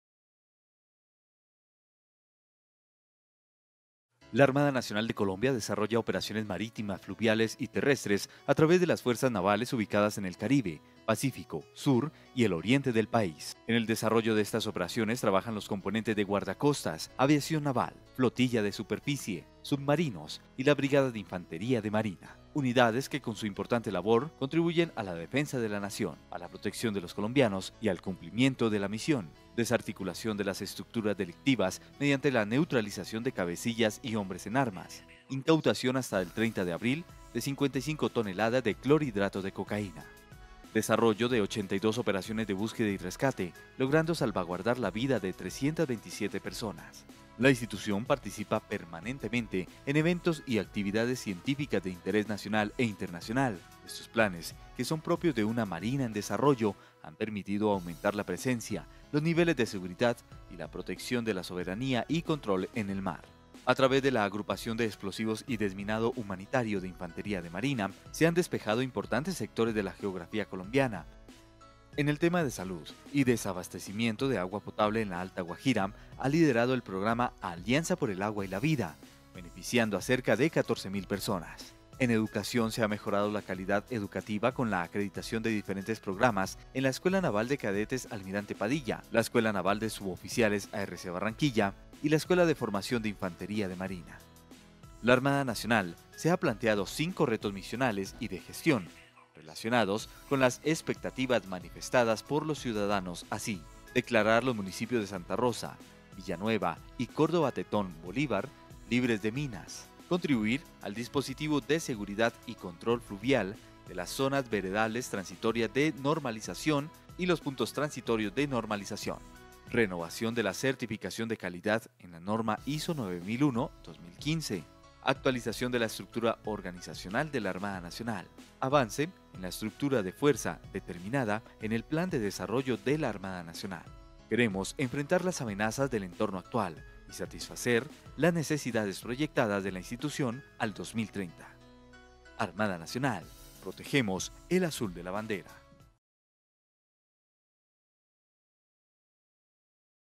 Armada Nacional presente para proteger el azul de la bandera
Armada Colombia​ desarrolla operaciones marítimas, fluviales y terrestres a través de las Fuerzas Navales ubicadas a lo largo y ancho del país.